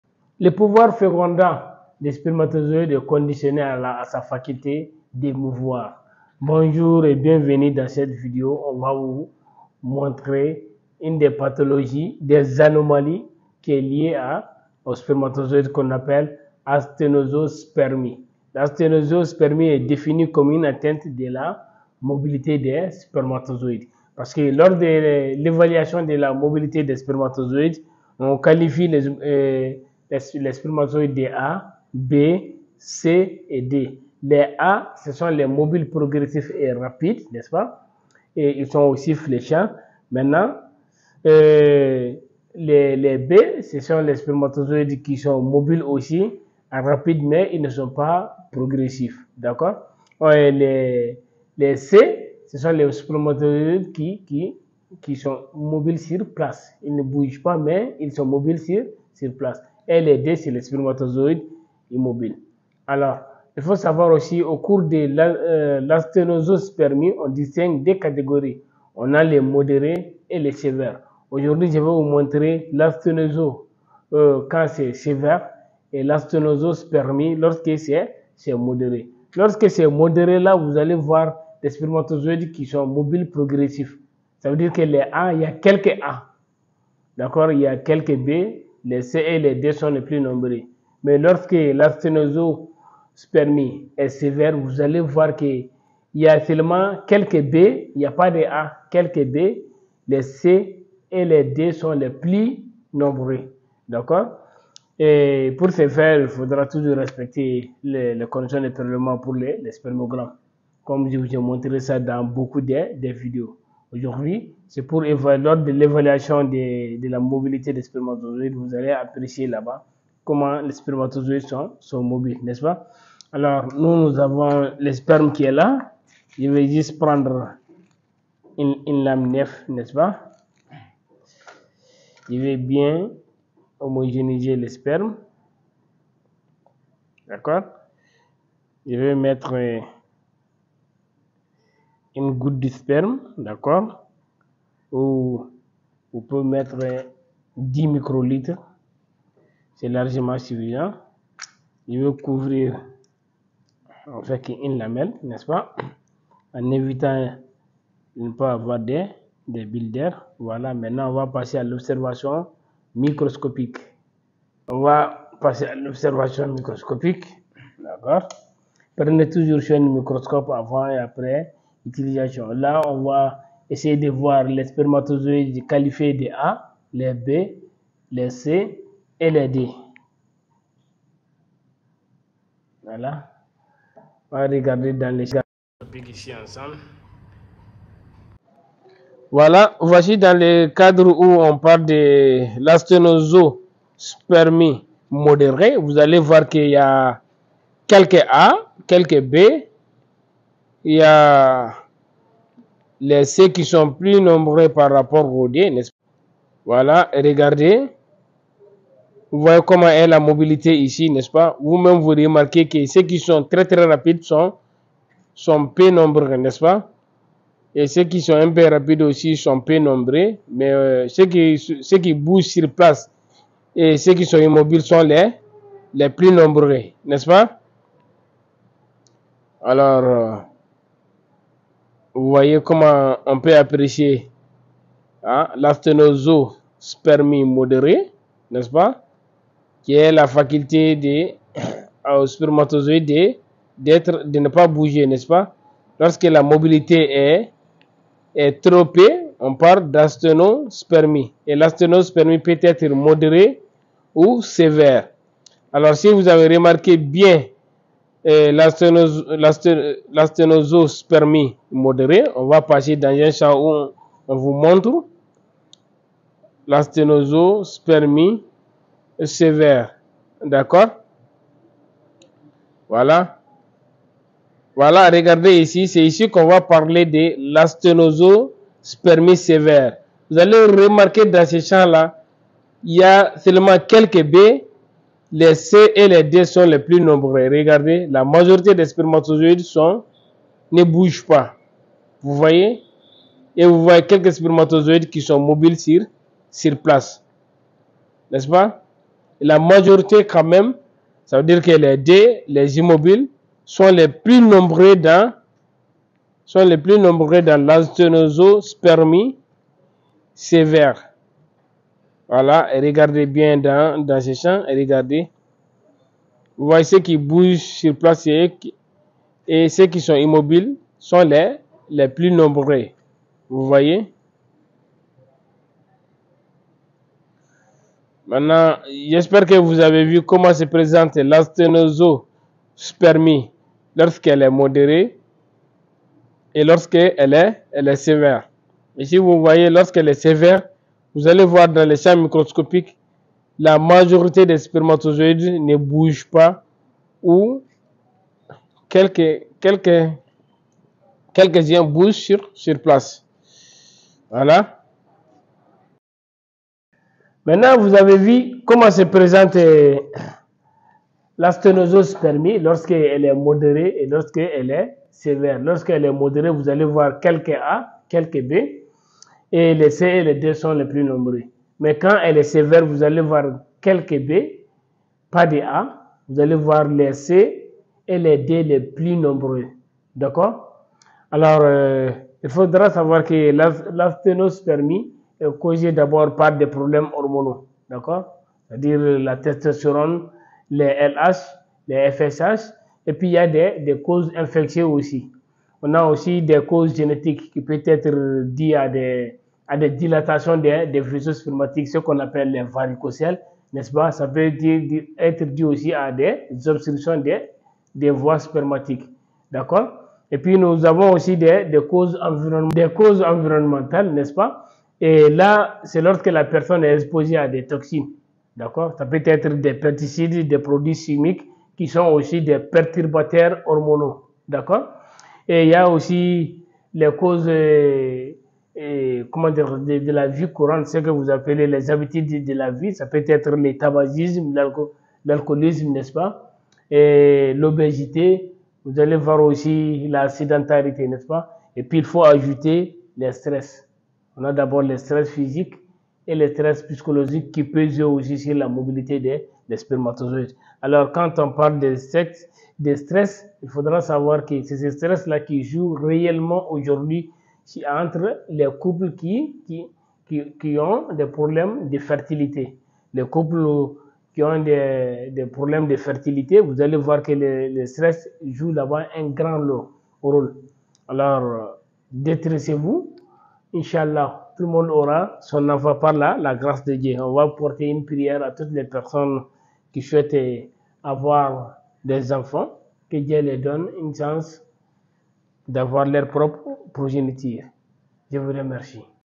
Asthénospermie modérée et sévère au microscope optique
L'asthénospermie ou asthénozoospermie est l'une des anomalies liées à l'infertilité masculine. Elle est caractérisée par une mobilité réduite des spermatozoïdes